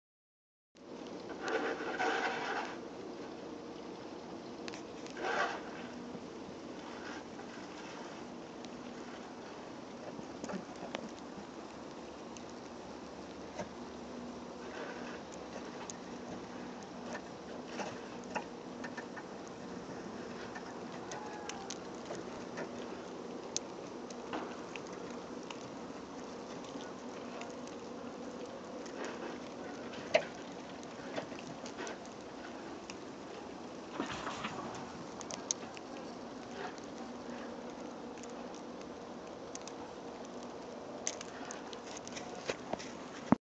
snow buns
angora rabbits discovering snow for the first time. near the end of the video, they hear a sound that startles them, so they head for home.